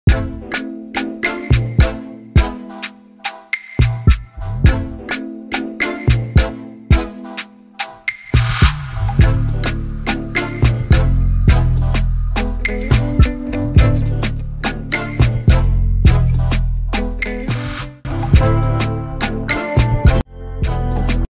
Automatic high frequency induction forging furnace for steel billet heating
Automatic high-frequency induction forging furnace for steel billet heating. The blanking mode in the video can be replaced by a robotic arm to cooperate with the work. to learn more.